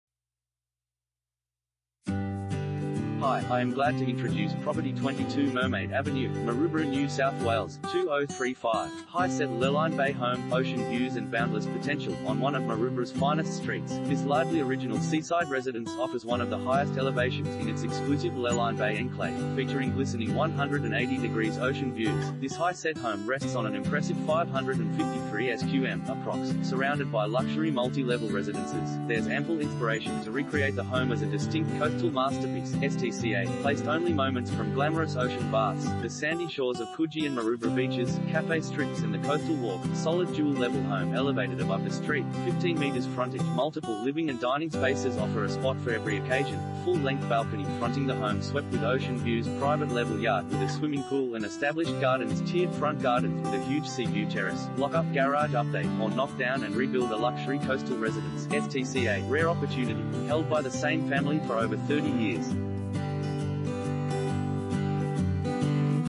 22 Mermaid Avenue, Maroubra NSW 2035 | AuPropertyReport.Com
22 Mermaid Avenue, Maroubra NSW 2035
Property Type: house
Bedrooms: 4
Bathrooms: 4
Car Space: 2
High-set Lurline Bay Home, Ocean Views &amp; Boundless Potential